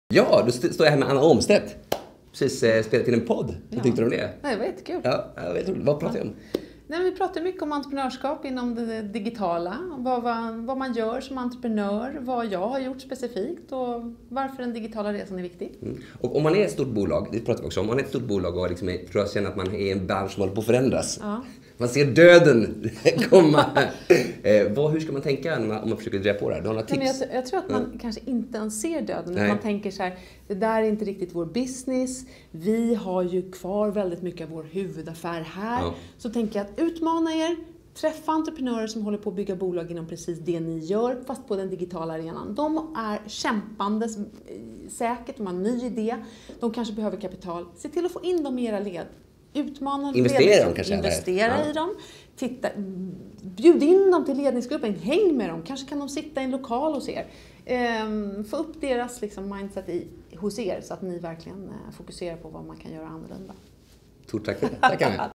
Techrekpodden avsnitt 28 - Anna Omstedt, VD och grundare på MedUniverse
Techrekpodden gästas av en riktig expert på digitala medier, nämligen Anna Omstedt. Anna har bland annat startat upp Tasteline, suttit i styrelsen för både Sveriges Radio och Hemfrid samt startat upp health tech-bolaget MedUniverse.

Digital utveckling står i fokus och Anna berättar om hur det är att vara entreprenör inom det digitala, den digitala resan för Sveriges Radio och kvinnor och kapital. Vi får också höra om bakgrunden till att Anna startade upp nätverket Pokerface, där 80 kvinnliga entreprenörer och företagare samlas och spelar poker, men främst för att utbyta erfarenheter av att starta och driva bolag.